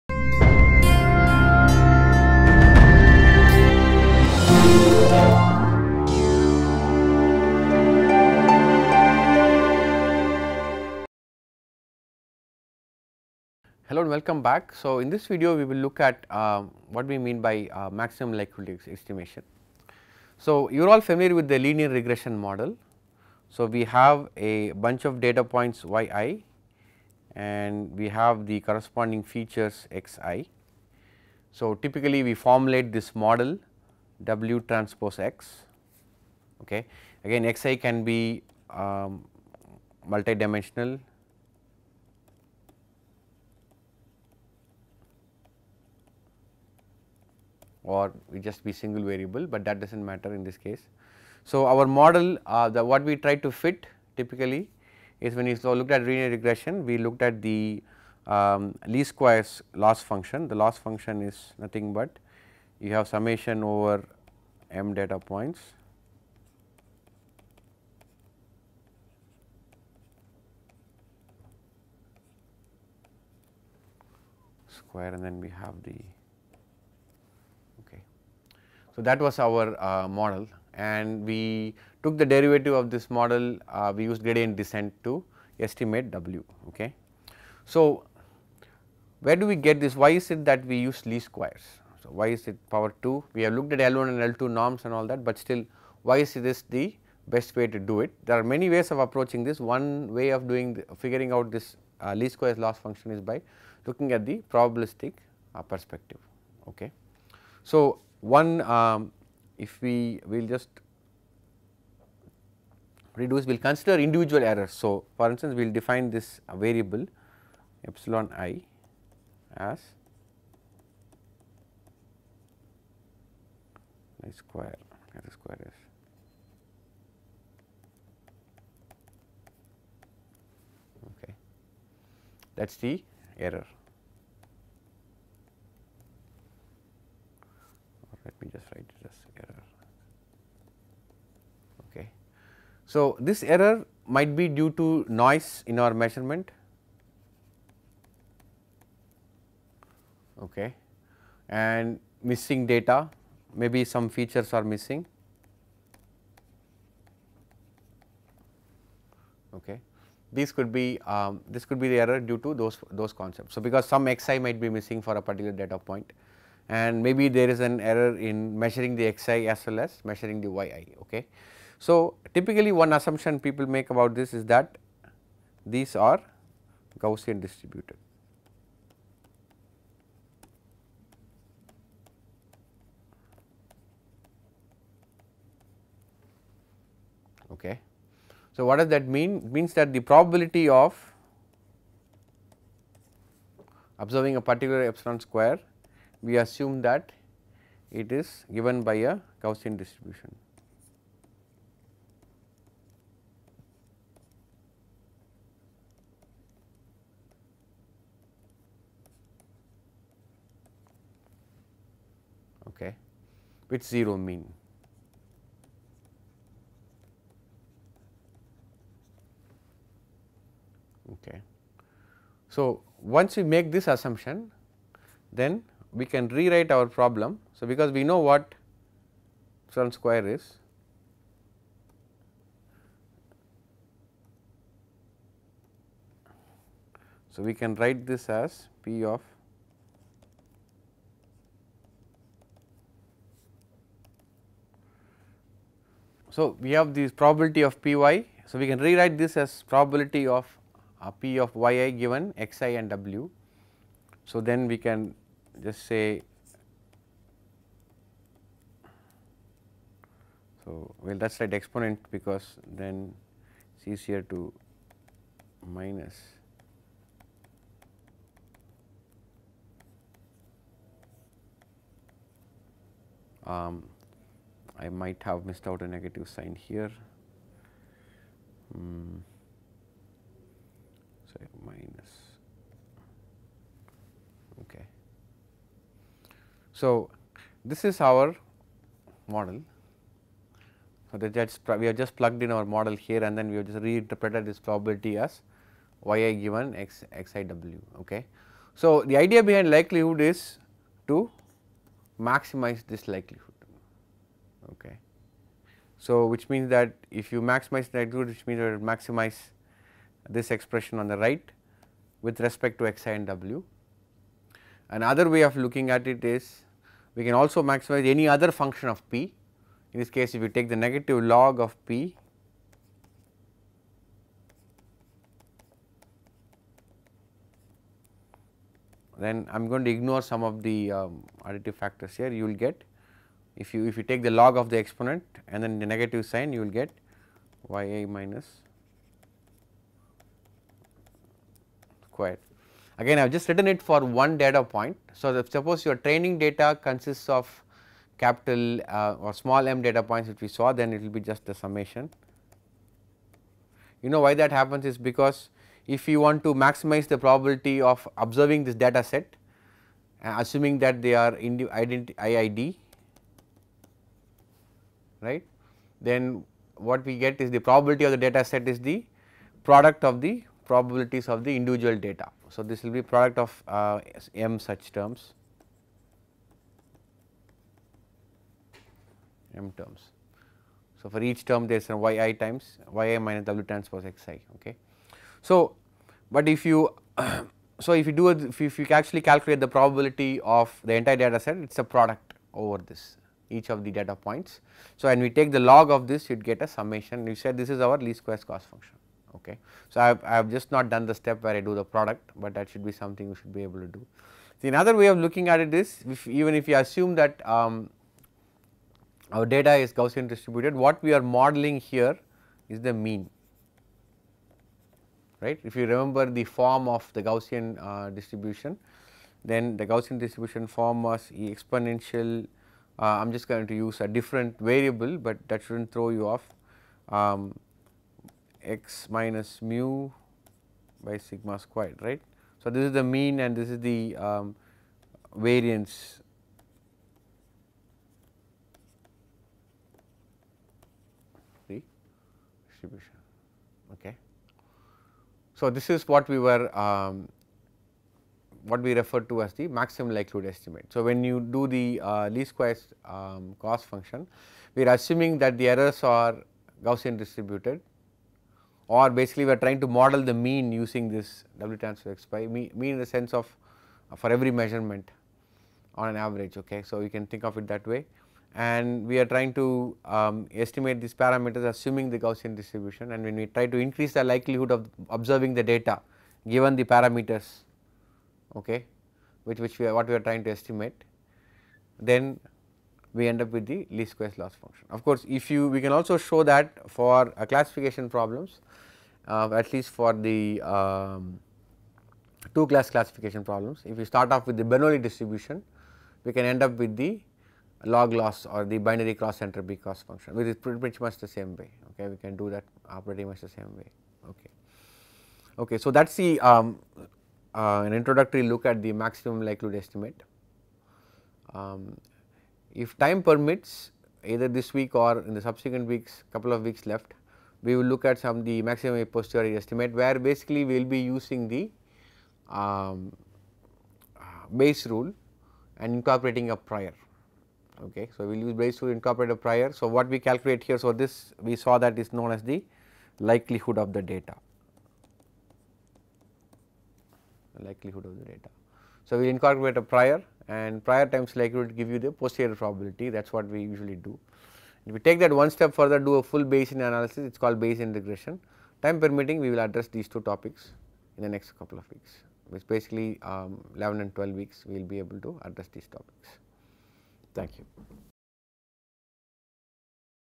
#88 MLE Intro | Machine Learning for Engineering & Science Applications
Welcome to 'Machine Learning for Engineering & Science Applications' course !

This lecture discusses maximum likelihood estimation, a method for estimating the parameters of a probability distribution. The maximum likelihood estimate of a parameter is the value of the parameter that maximizes the likelihood of the observed data.